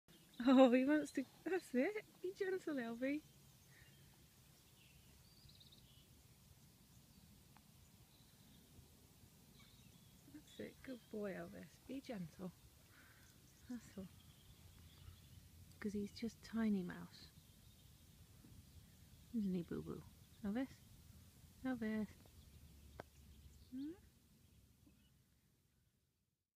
Dog kisses mouse
Elvis, our goldie flat coat cross, found a mouse in the garden and was transfixed by the little fella.